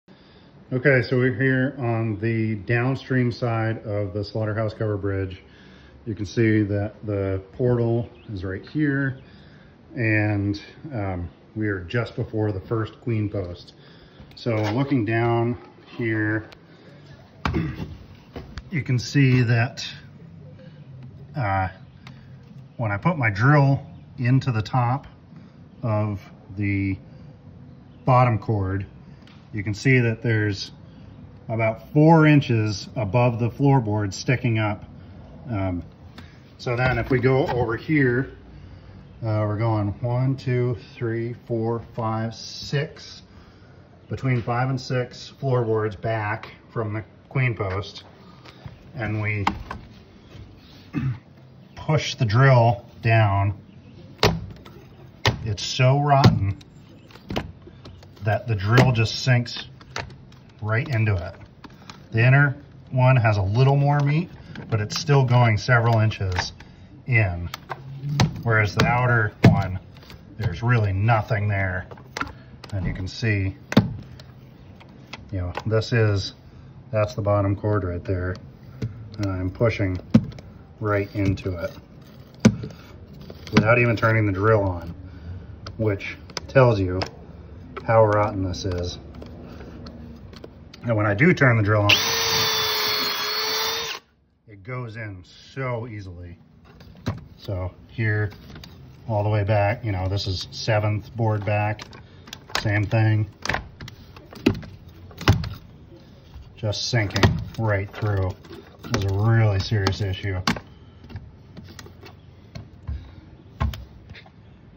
Slaughter House covered bridge downstream chord inspection, 7/16/25, Northfield Vt
This is a visual explanation of the rot that we found in the bottom chord of the bridge.